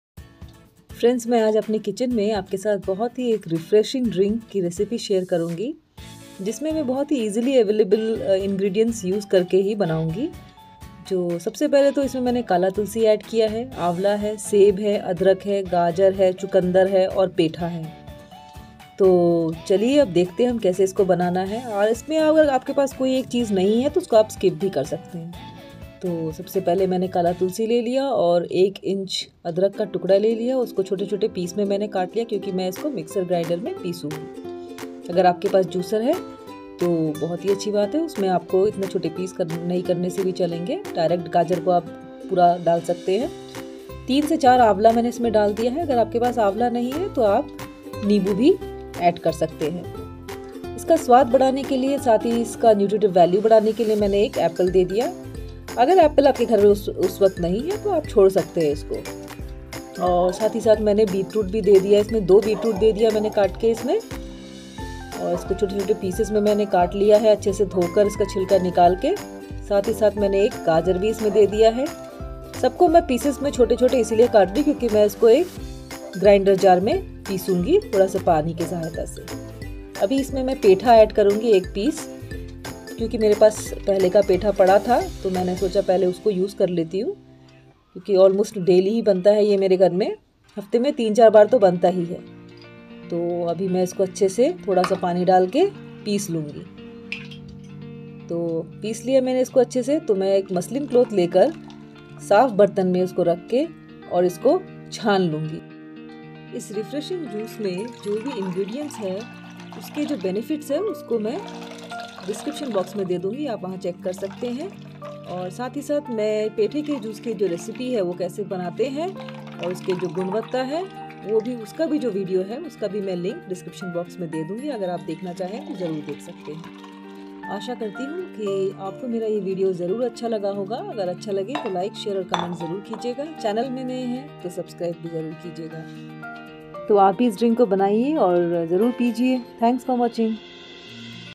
weight loss drink/Immunity booster drink/Drink for long life/Increases hemoglobin levels
link of Ash gourd juice

health benefits of ginger

Ginger isn’t a cure-all and is not a substitute for bona fide medical treatment. However, research has shown it may have some health-boosting properties.

Eases nausea and vomiting
Relieves pain
Lowers blood sugar
Improves brain health
Decreases migraine pain
Reduces arthritis inflammation
Helps with weight loss
Lowers cholesterol levels
Prevents infection
Lowers blood pressure
Faster muscle recovery
Reduces menstrual bleeding

benifits of Kali Tulsi

Natural Immunity Booster: ...
Reduces Fever (antipyretic) & Pain(analgesic): ...
Reduces Cold, Cough & Other Respiratory Disorders: ...
Reduces Stress & Blood Pressure: ...
Anti-cancer properties: ...
Good for Heart Health: ...
Good for Diabetes Patients: ...
Useful in Kidney stones & Gouty Arthritis

 Benefits of Drinking Amla Juice

Promotes immune function
Amla juice is a great source of vitamin C which is a water-soluble vitamin that acts as an antioxidant
Enhances liver health
Supports healthy digestion
Promotes heart health
May increase hair growth
Improves kidney health.

Benefits of Apple Juice :

Enhances skin.
Aids weight control.
Aids a healthy heart.
Keeps liver healthy.
Cures constipation.
Good for diabetes.
Hydrates the body
Prevents anemia.

 benefits a carrot

They improve eyesight
The lower the risk of cancer
They help with heart health
The vitamin C present in carrot would boost the immunity system
Carrots helps in controlling diabetes
Carrots have calcium and vitamin K which improve bone health.
Helps lower blood pressure.

benifits of beetroot juice

Beet juice may help lower your blood pressure
Improves exercise stamina
May improve muscle power in people with heart failure
May slow the progression of dementia
Helps you maintain a healthy weight
May prevent cancer.
Good source of potassium.
Good source of other minerals.

10 Health Benefits of Ash Gourd

Ash Gourd aids in better digestion.
Ash Gourd improves lung health.
Ash Gourd increases energy levels.
Ash Gourd treats ulcers naturally.
Ash Gourd contains anti-coagulant properties. ...
Ash Gourd provides a cooling effect.
Ash Gourd relaxes the body.
Ash Gourd regulates excretory system
Nami ka kitchen